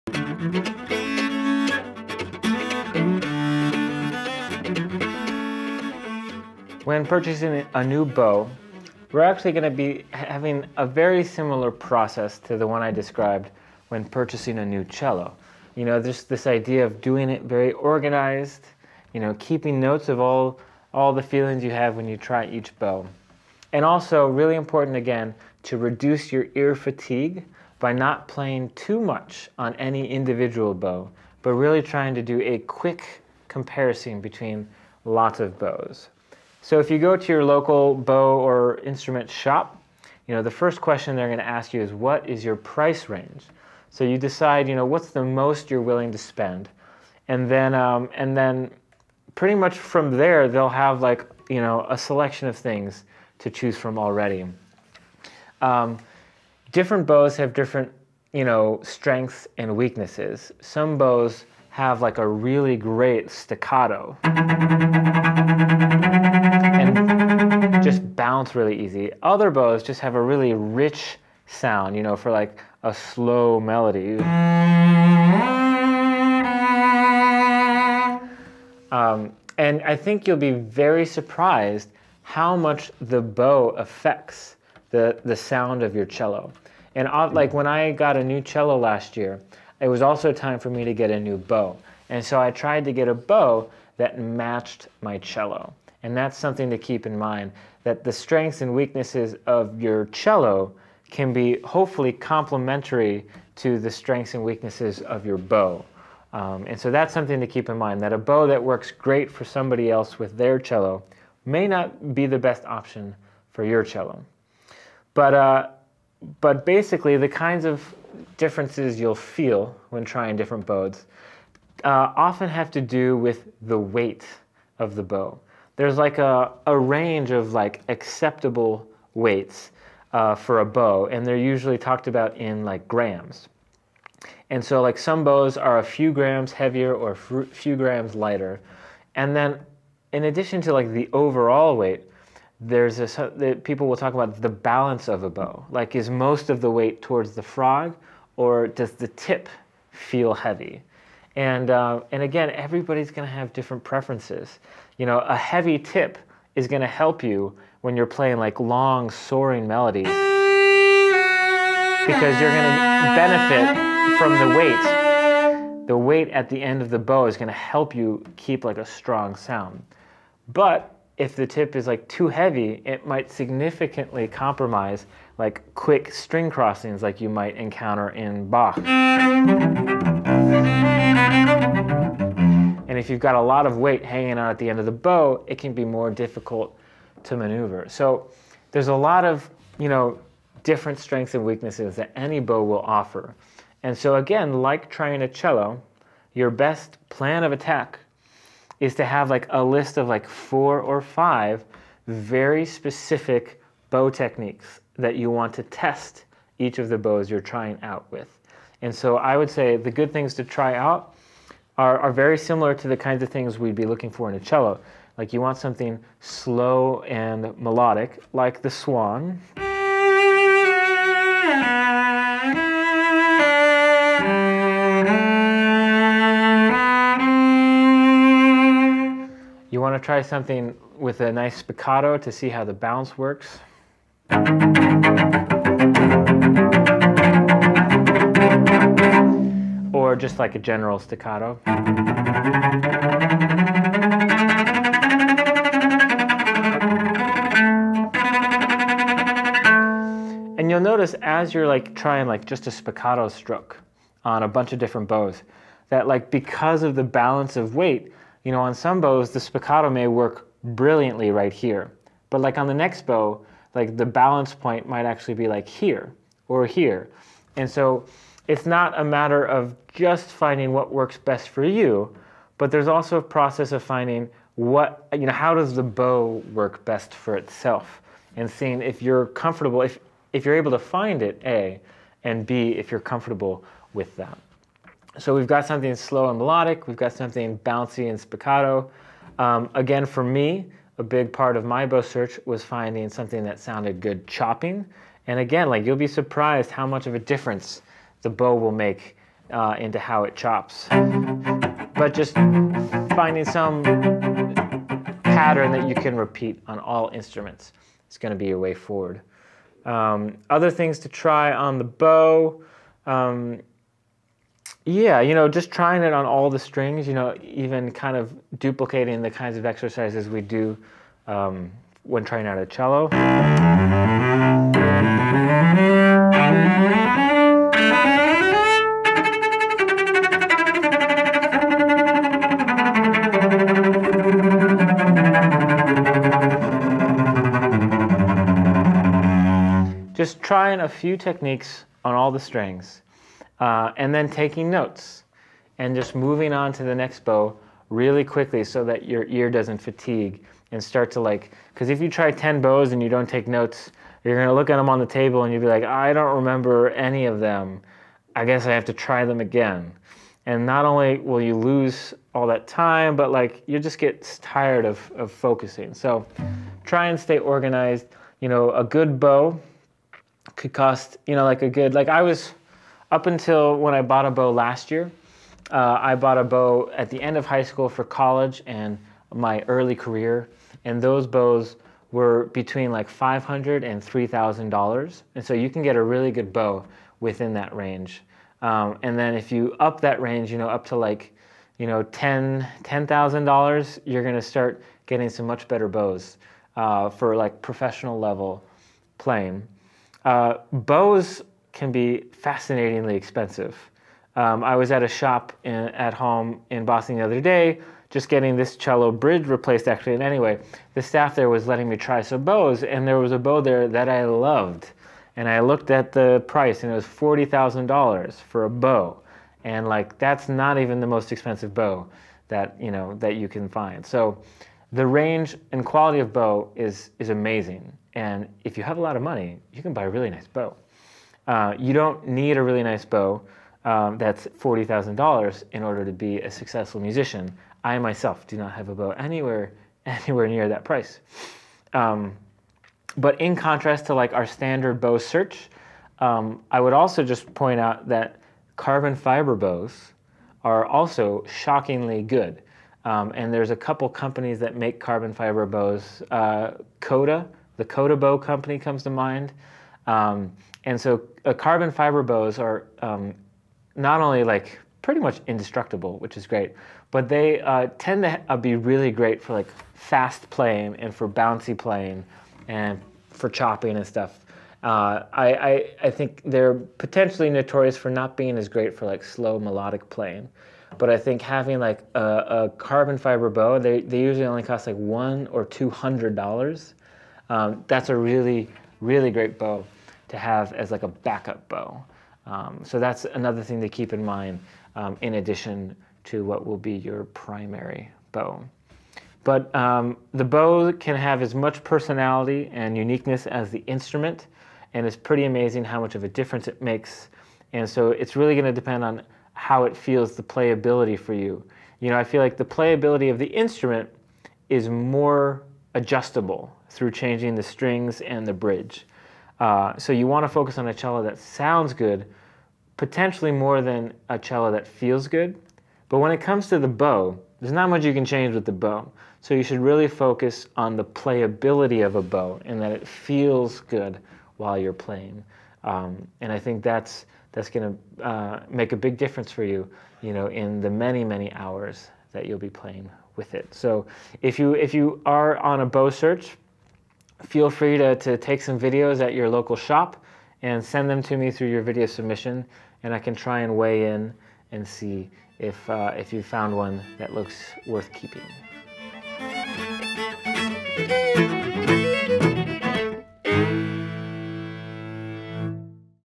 How to Choose a Cello Bow
Mike Block, ArtistWorks Cello Instructor, shows you how to choose the perfect cello bow. Join ArtistWorks online Cello school to improve your playing in no time